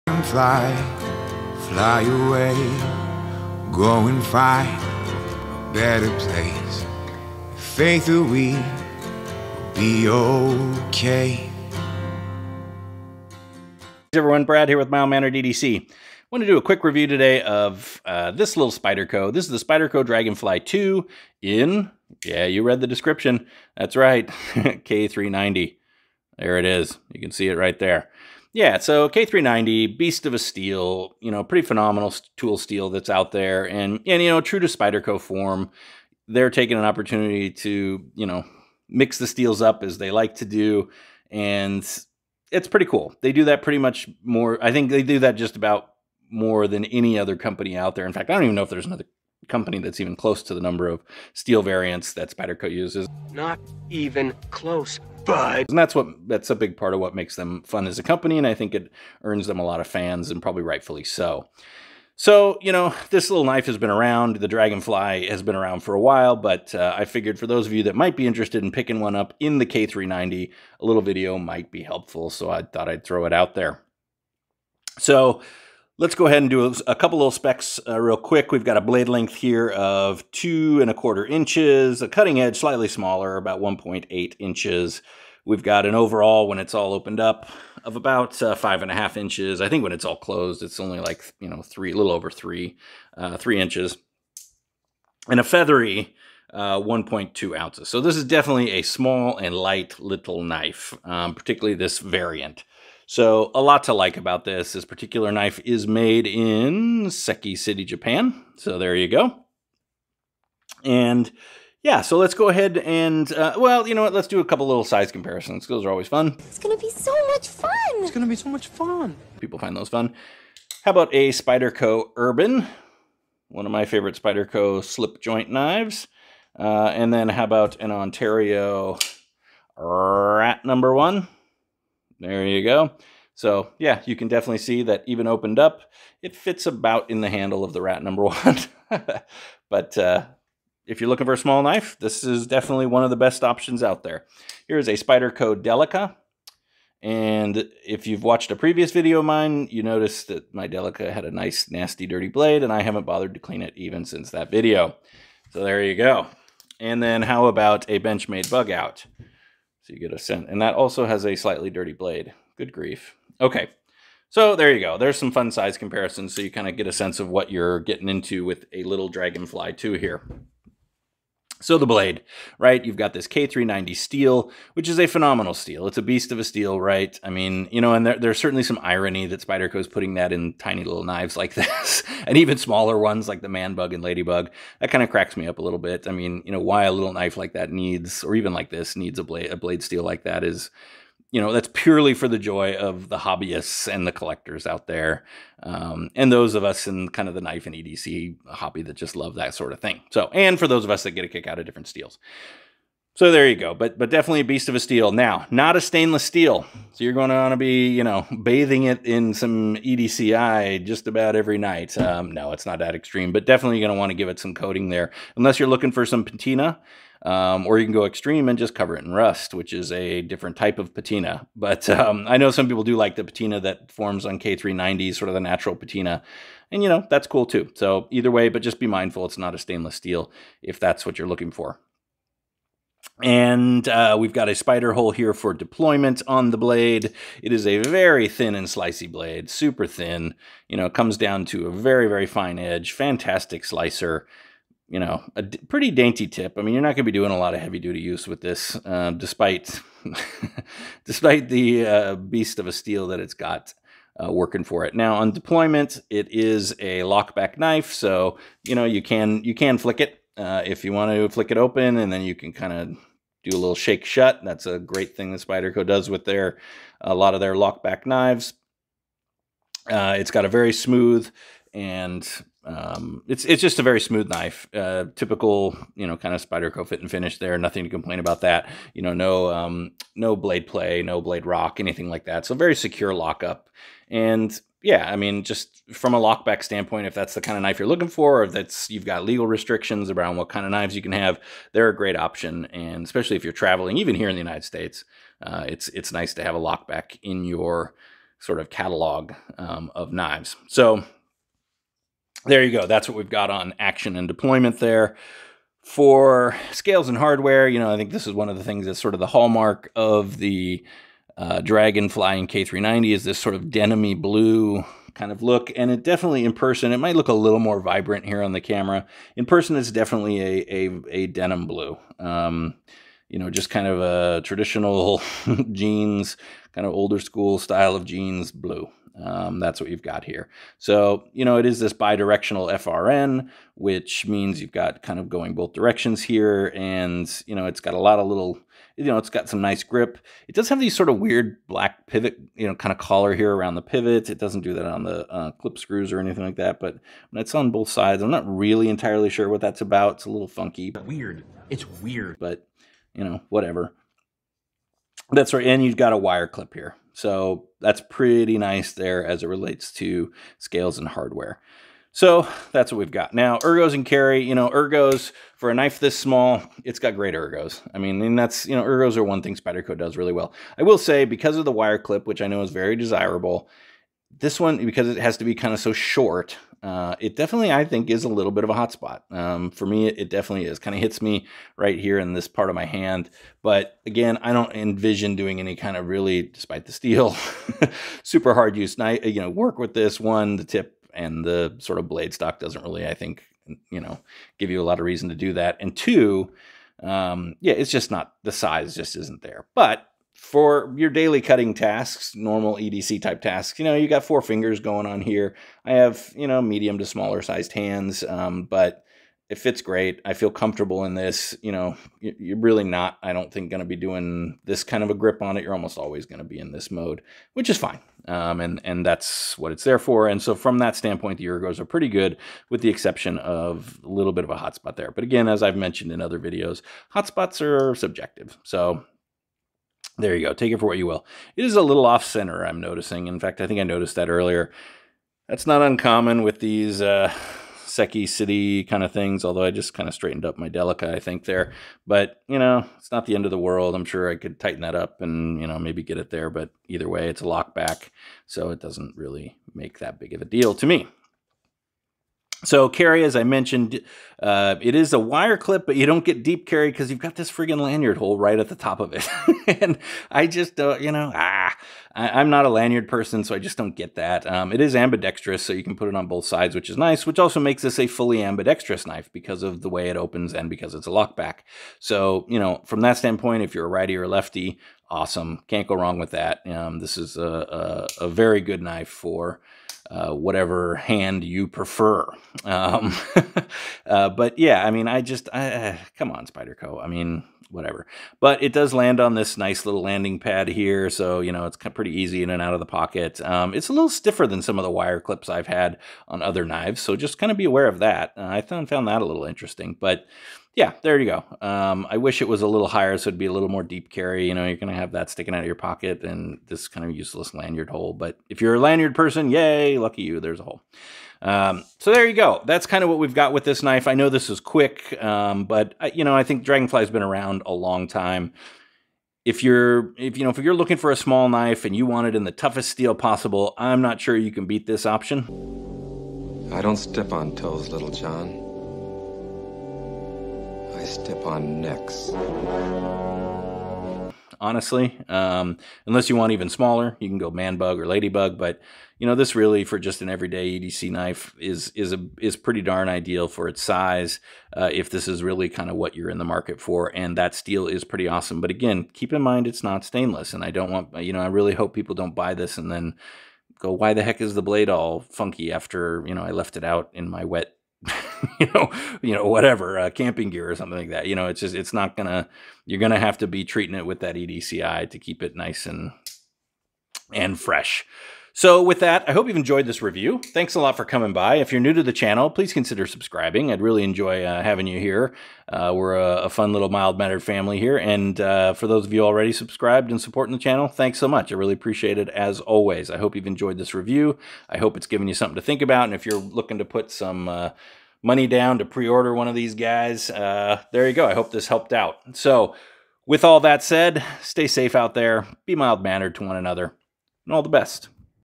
The Spyderco Dragonfly 2 in K390 Steel: A Mild Mannered Quickie Review
If you are looking for a strong, slicey and small EDC pocketknife that is legal in a lot of places, the Dragonfly 2 is one of the best on the market... add a crazy beast of a steal like K390 and you've got one special little knife.

Music purchased and downloaded from Amazon Music.

Stay safe out there, be mild mannered to one another and all the best!